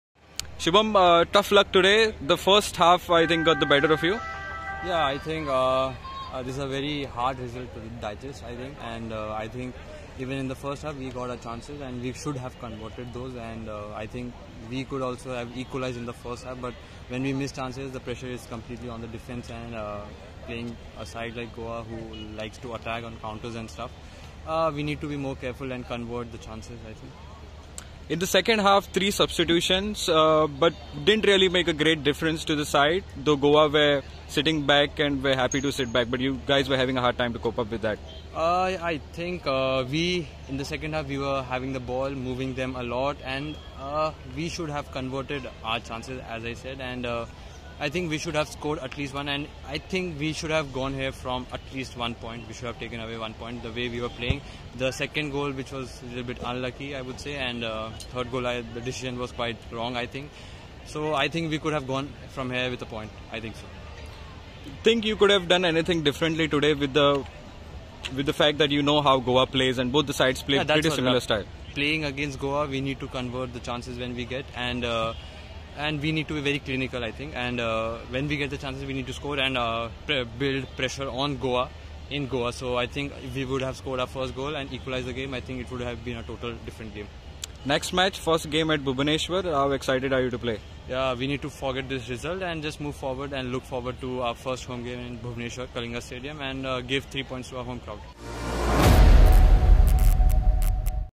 post match interview subham sarangi🏆🏆⚽⚽🏃odisha fc team
-please support Odisha football club